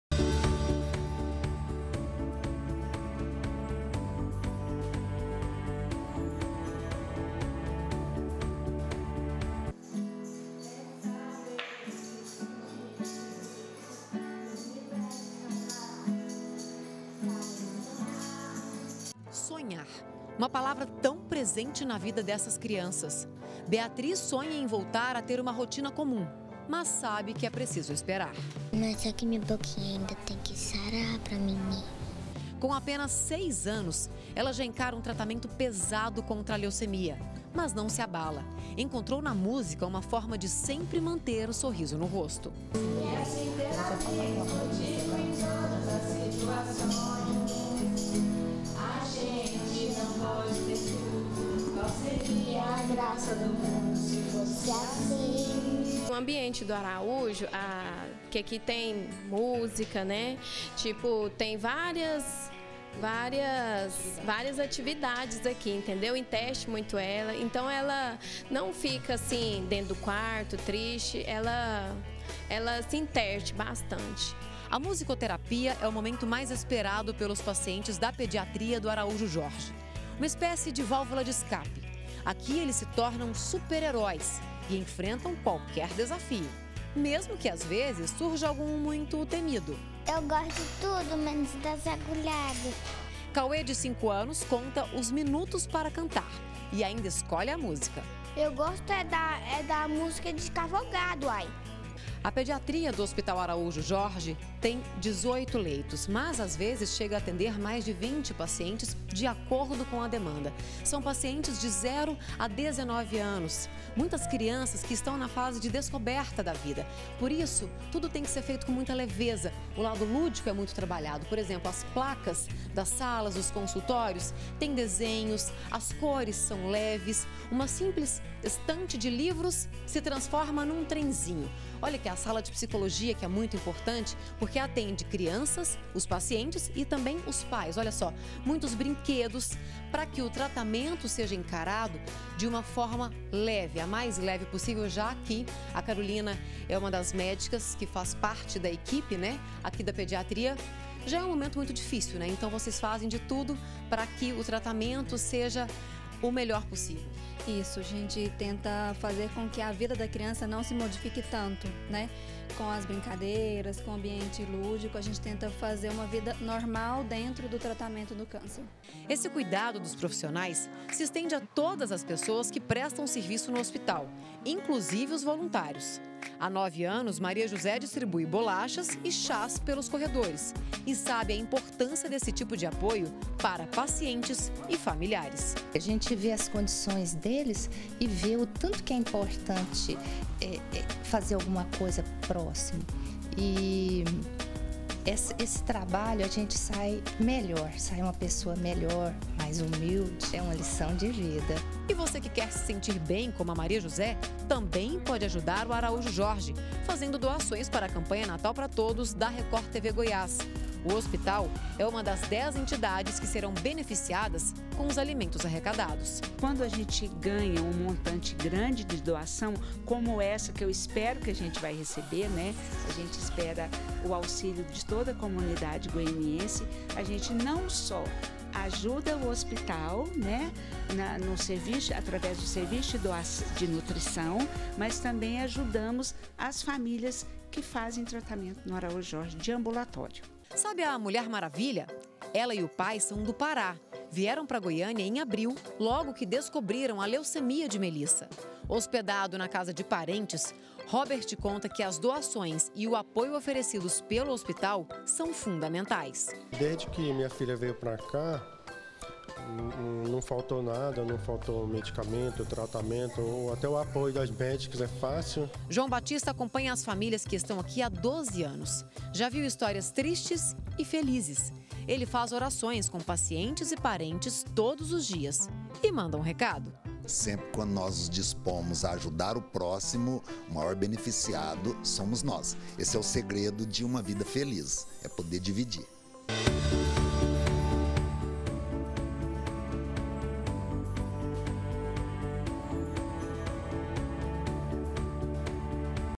GA - Voluntários alegram o natal de crianças doentes - 05-12-2017
O hospital Araújo Jorge é um dos pontos de arrecadação da campanha "Natal pra Todos", da Record TV Goiás. Mesmo diante da dor e do tratamento difícil, os pacientes encontram nos voluntários e na equipe médica um pouco de esperança. A repórter Manuela Queiroz esteve no hospital.